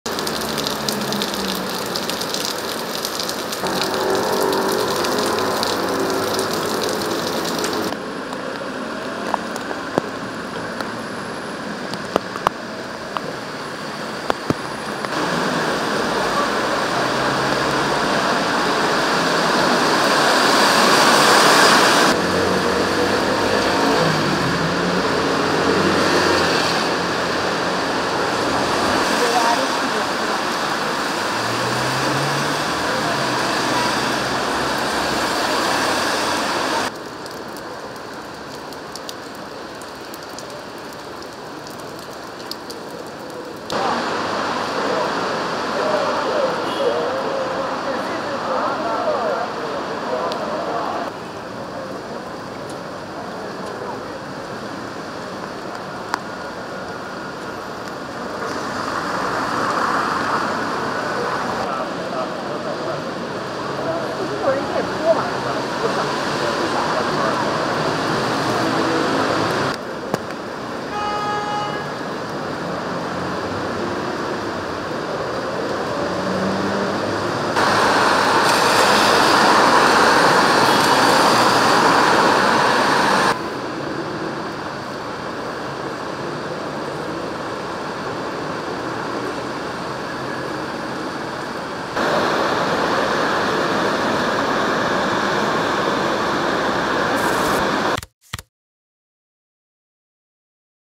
Activada la alerta naranja por fuertes lluvias en Barcelona
La semana en Barcelona ha comenzado con fuertes lluvias que amenazan con quedarse al menos hasta mañana. Protección Civil ha activado la alerta del Plan especial de emergencias por inundaciones (Inuncat) por lluvias, que pueden superar los 20 litros por metro cuadrado en media hora. Se prevén acumulaciones de hasta 100 litros por metro cuadrado en 24 horas. Los chubascos pueden ir acompañados de tormentas, además de de fuertes rachas de viento.